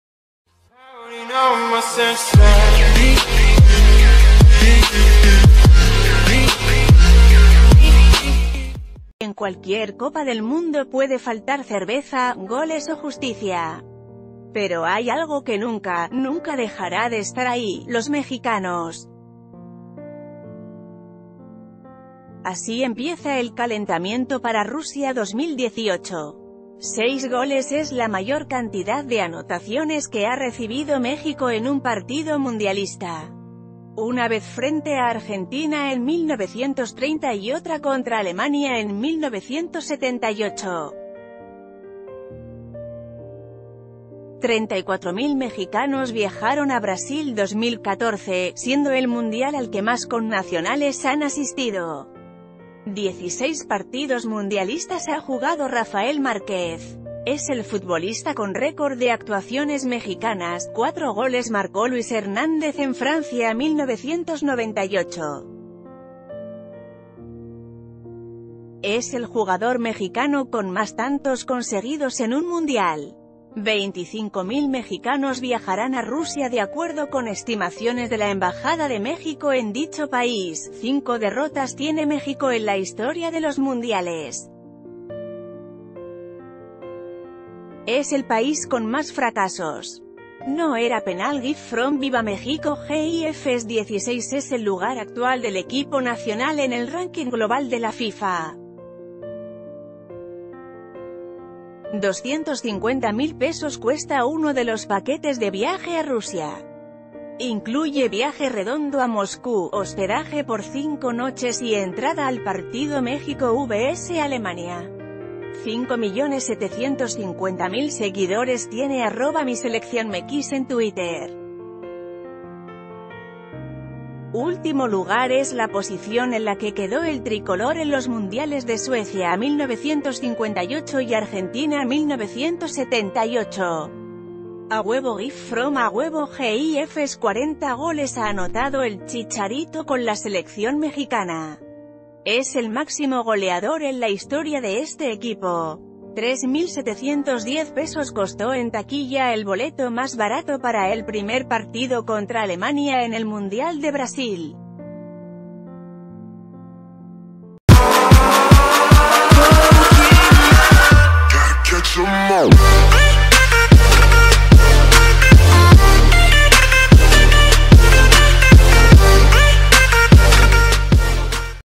Mexicanos en los Mundiales, así nos vamos en números
En cualquier Copa del Mundo puede faltar cerveza, goles o justicia. Pero hay algo que nunca, nunca dejará de estar ahí: los mexicanos. Así empieza el calentamiento para Rusia 2018. 6 GOLES es la mayor cantidad de anotaciones que ha recibido México en un partido mundialista. Una vez frente a Argenti...